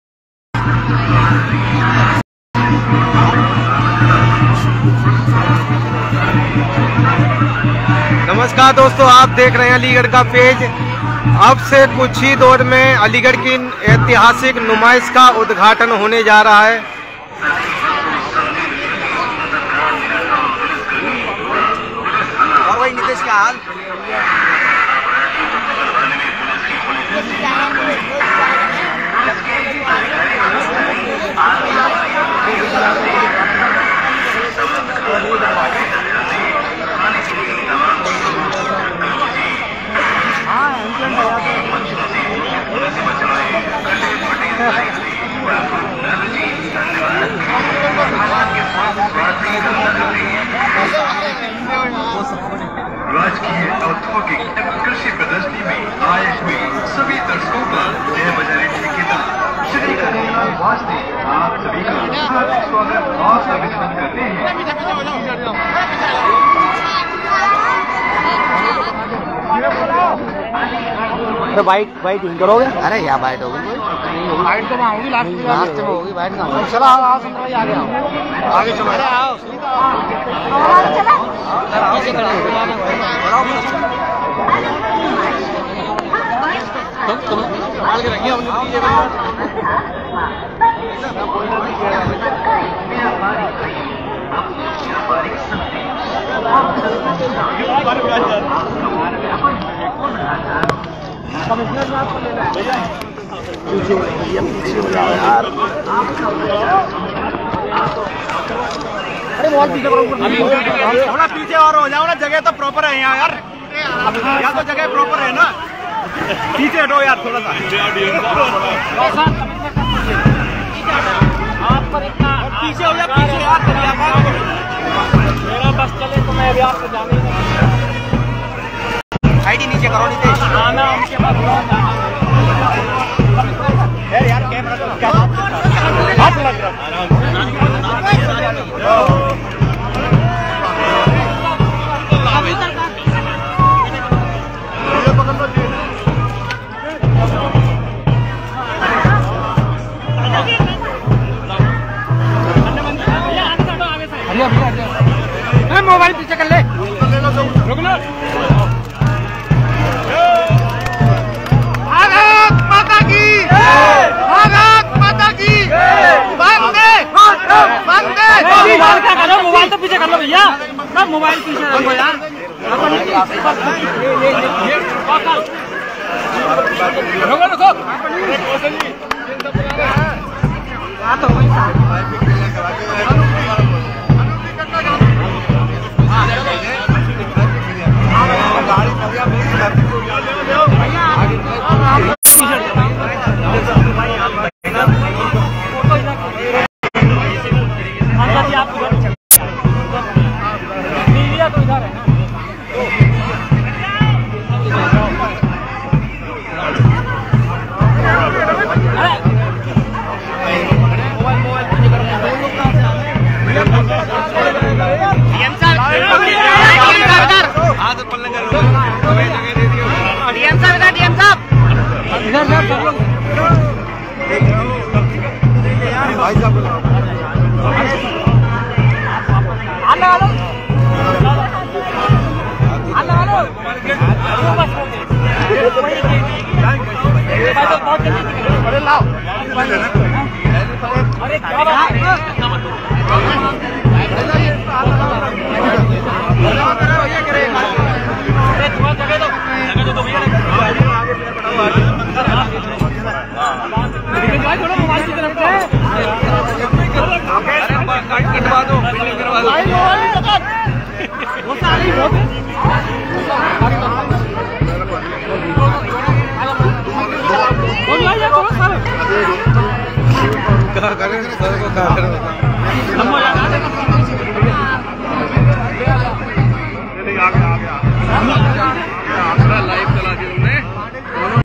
अलीगढ़ नुमाइश का कैबिनेट मंत्री सुरेश खन्ना ने किया उद्घाटन, कबूतर उड़ाकर की Aligarh Exhibition शुरू
aligarh News
aligarh numaish
aligarh video
aligarh mahotsav